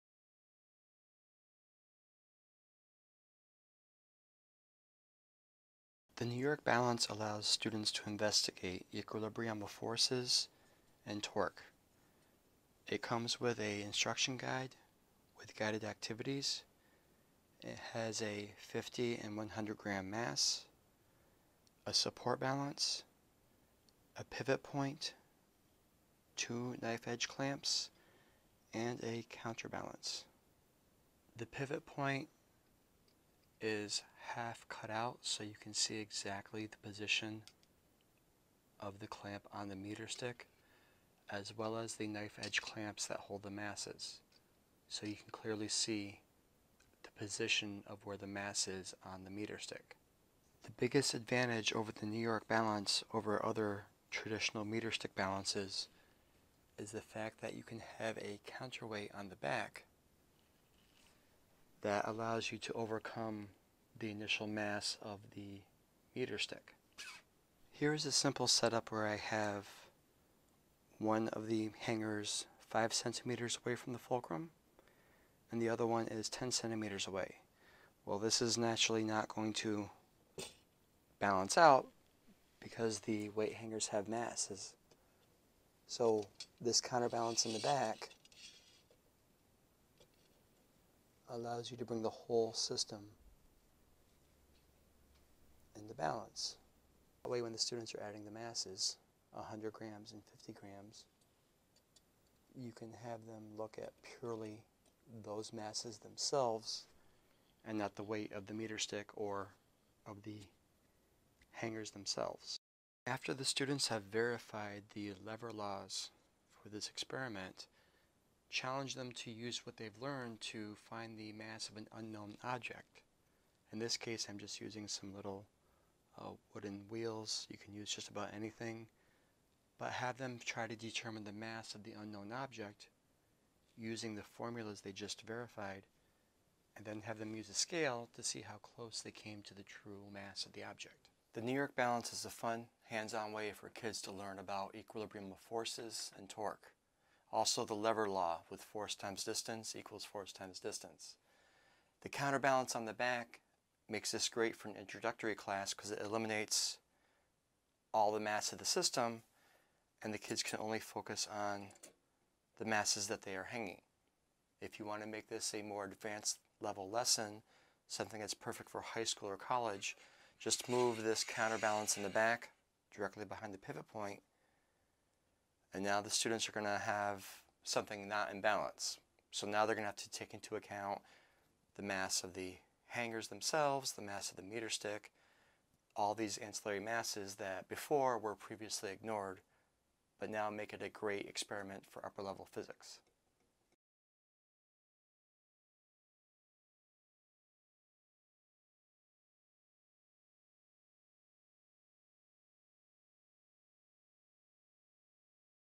Understanding levers (NYB001)
Demonstrate a variety of physics concepts, including equilibrium of forces and the law of levers, with the New York balance.

This set includes:
* 1 demonstration balance support
* 3 knife-edge lever clamps
* 1 half-meter stick
* 1 hooked weight, 50g
* 1 hooked weight, 100g
* Activity guide